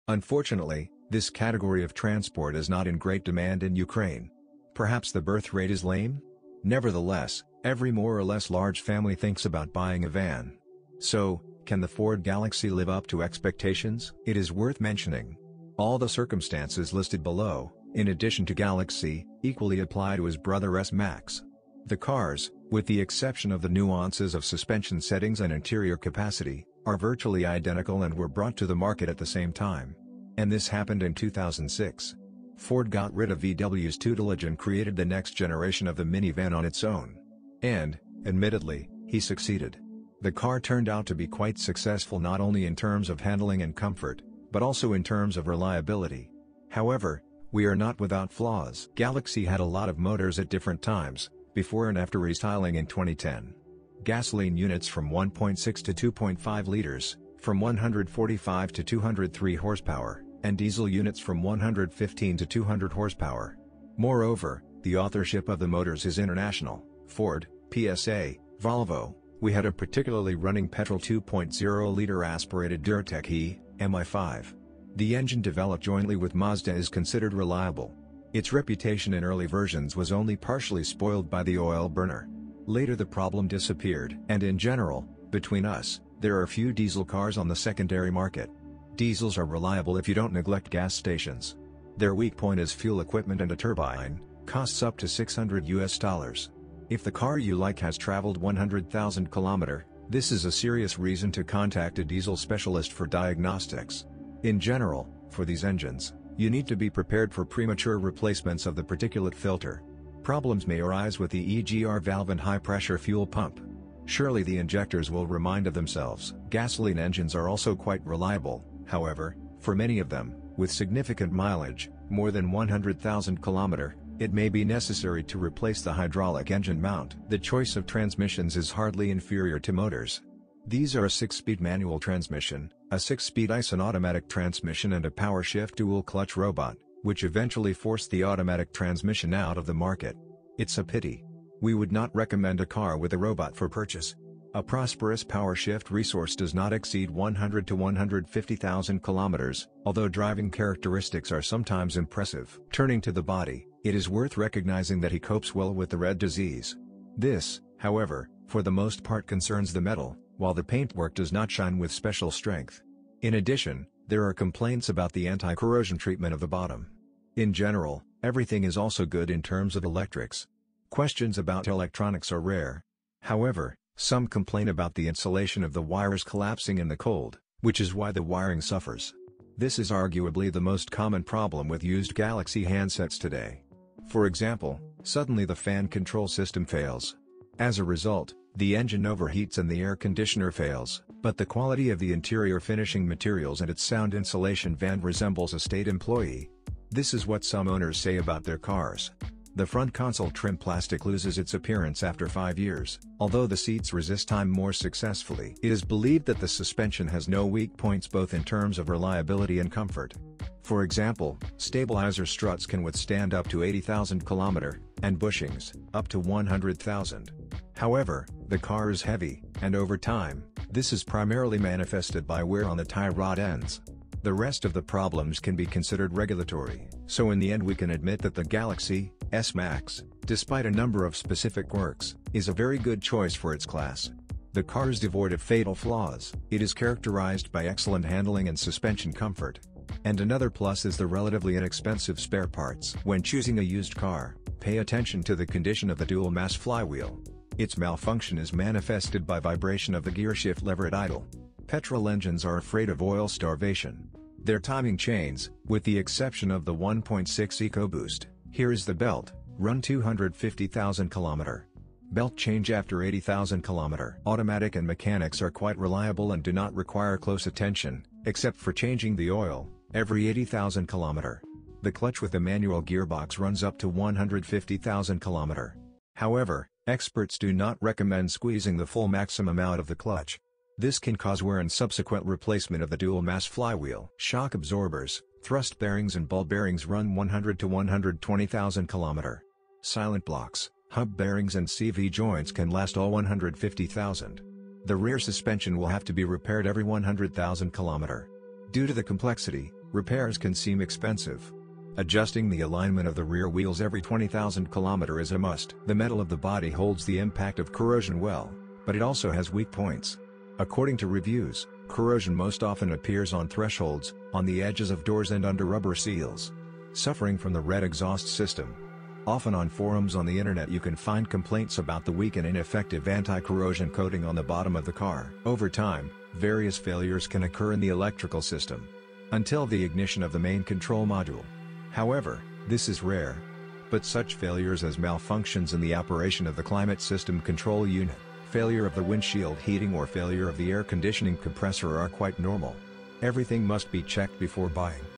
Ford Galaxy 2006 - 2015 - high Miles review
Common Issues Ford Galaxy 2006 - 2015 generation